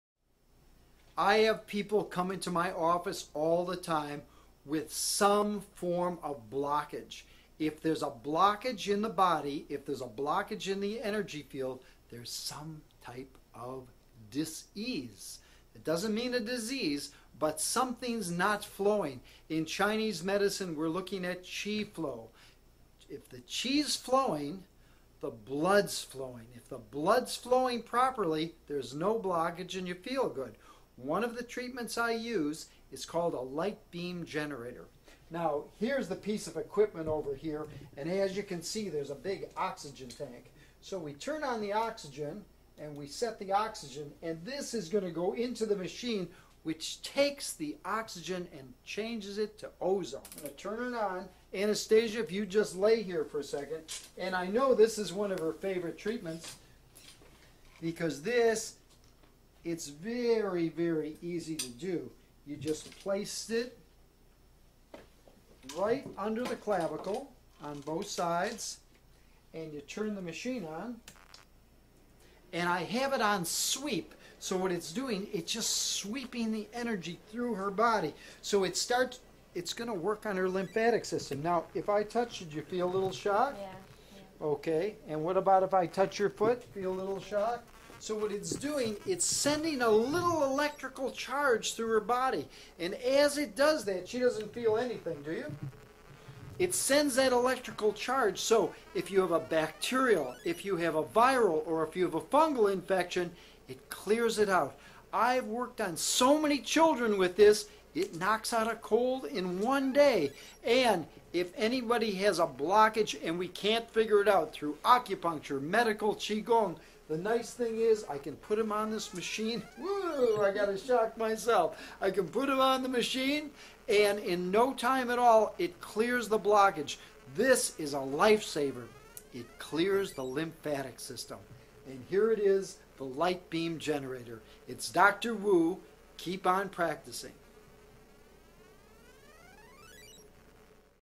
Detox treatment to get rid of toxins in your body
We are getting hit with so many
Environmental toxins-in the air
and the chemicals in our
food, water, workplaces and the so called
"New-and--Improved" products we put into
our bodies all expose us to environmental
toxins. If you want to stay healthy it takes
both of us working together to achieve one goal.
Your Perfect Health.